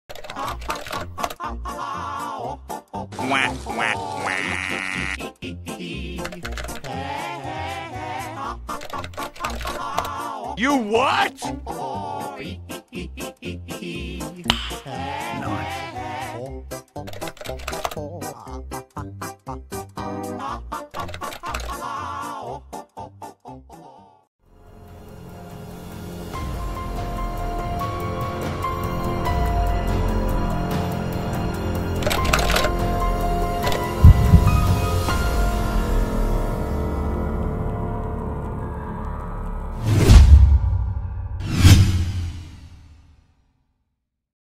I made a new intro due to content id
well I knew it was coming but now I had to change it hopefully you guys like the new intro song